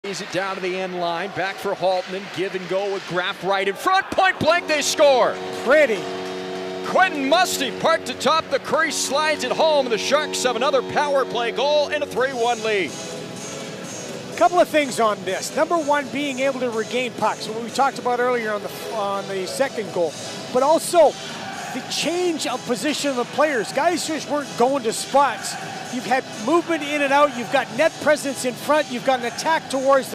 Quentin Musty Goal
Questin Musty scores at the Rookie Faceoff on September 15, 2024.